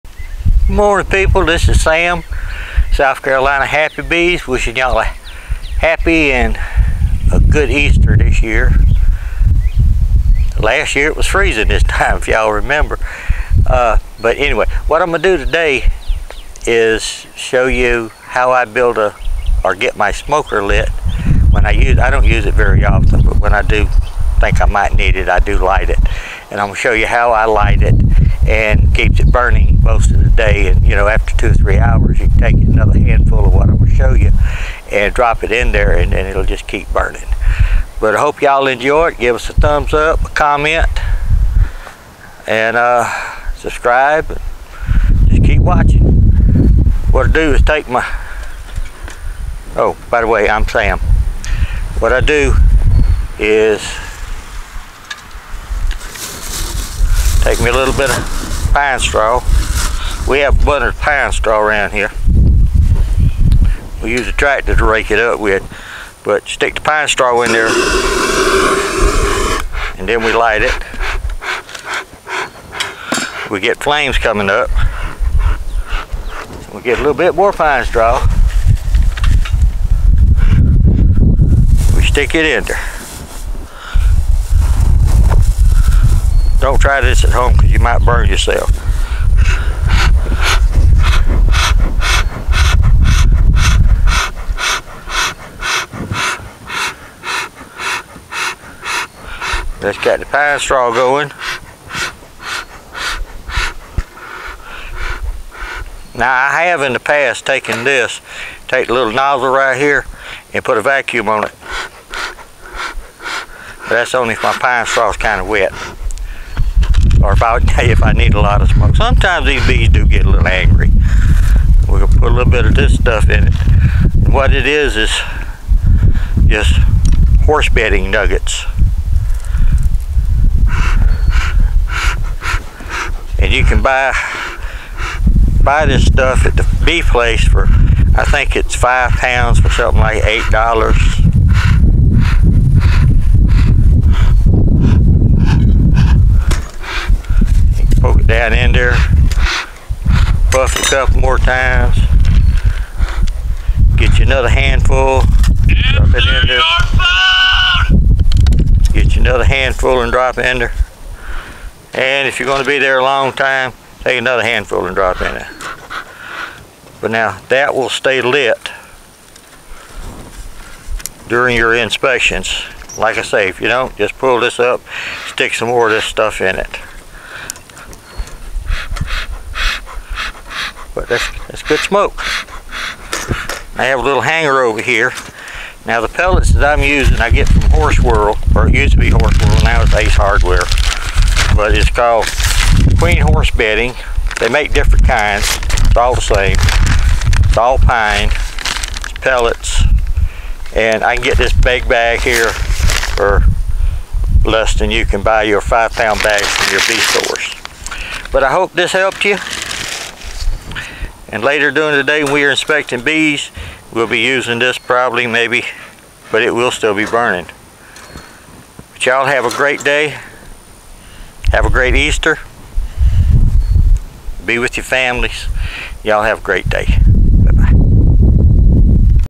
How to light a smoker and keep it smoking for your inspections
This is how I light my smoker to last longer for inspections.